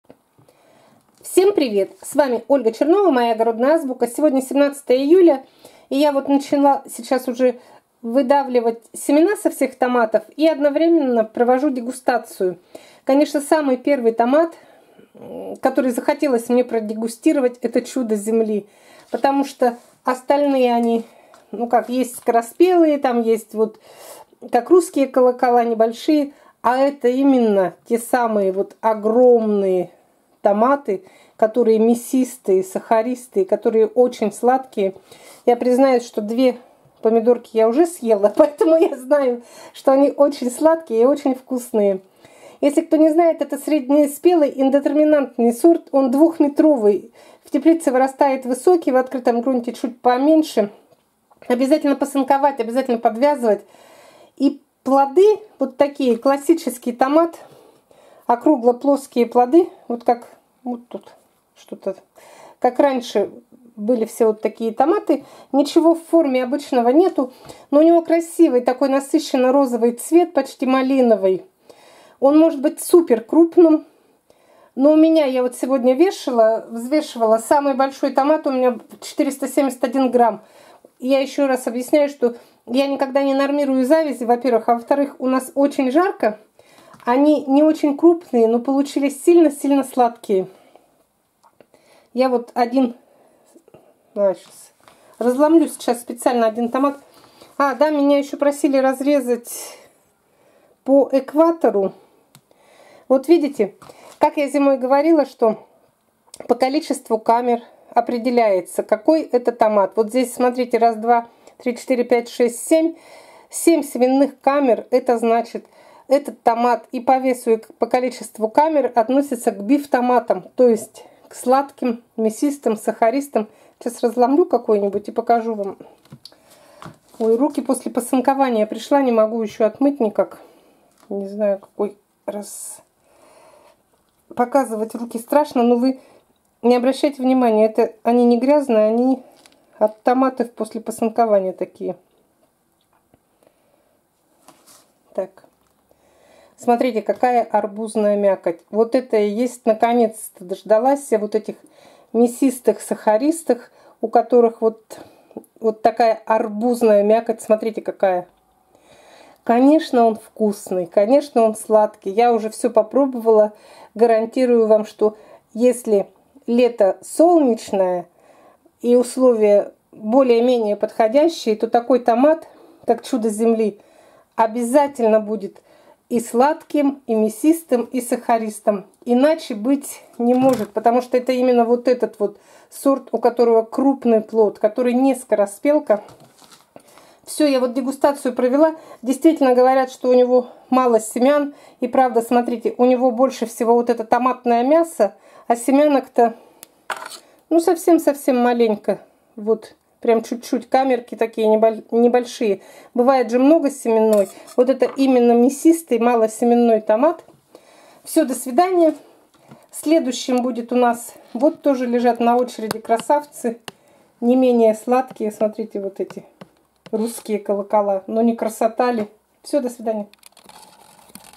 ТОМАТ ЧУДО ЗЕМЛИ. ДЕГУСТАЦИЯ.19 июля.
Продолжаю дегустацию томатов. Сегодня это будет томат Чудо земли.